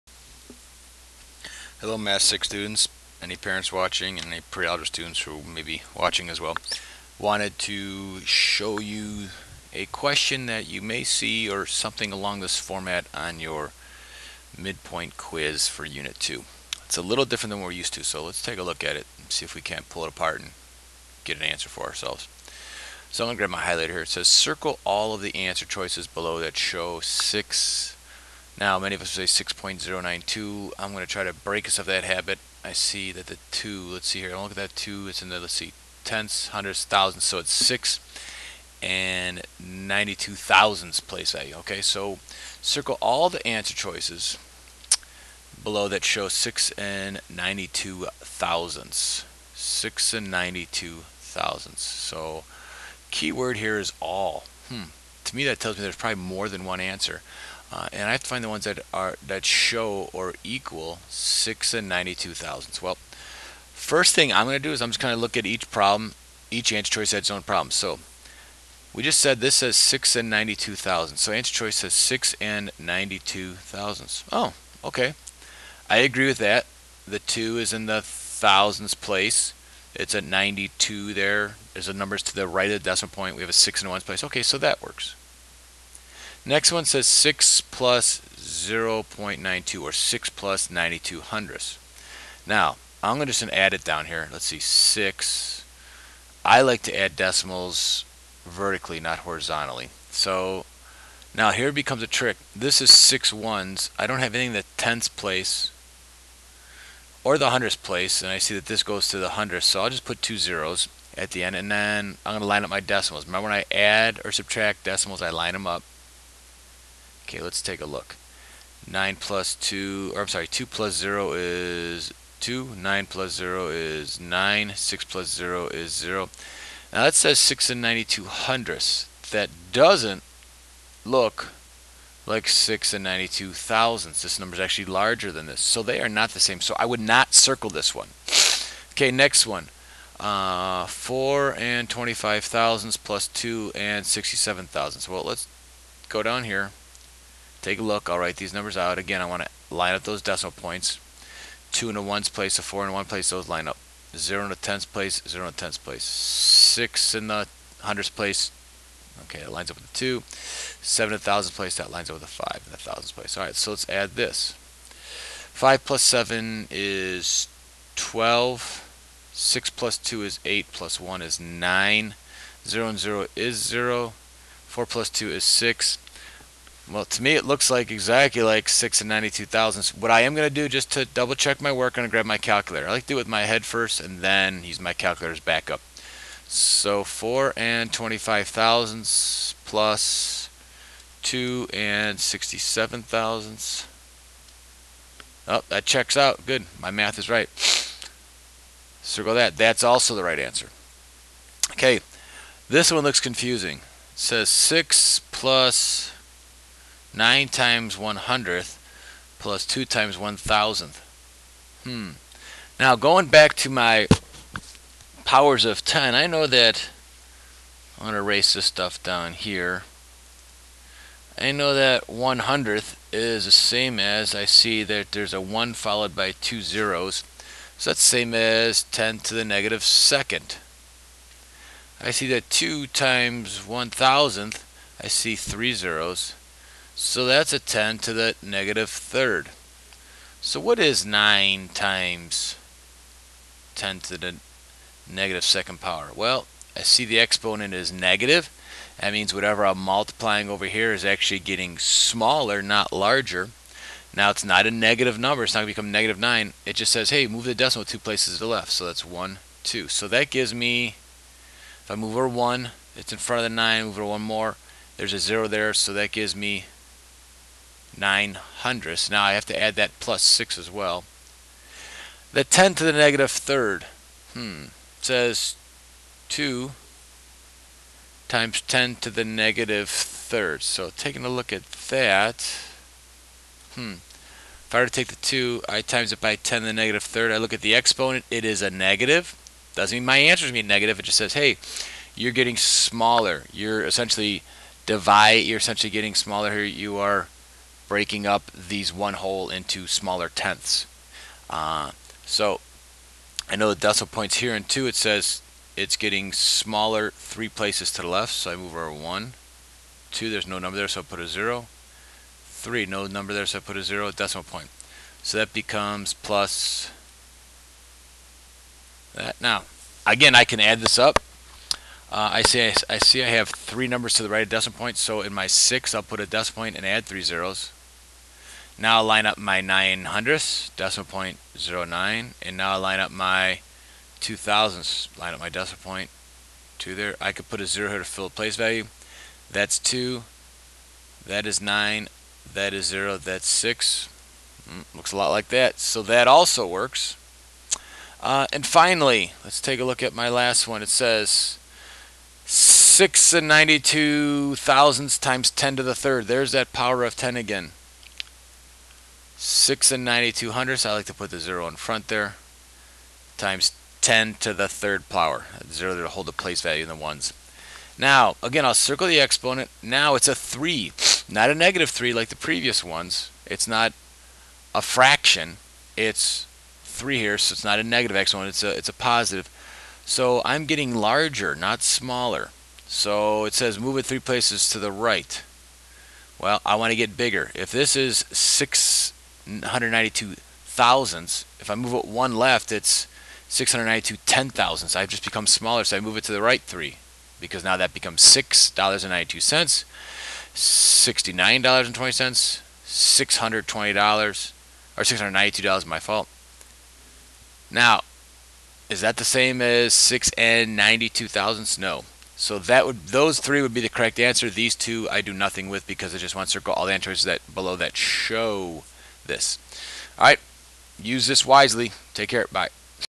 Math Six: Unit Two Quiz Prep
This is a quick video on how to answer multiple choice questions when there is more than one correct answer.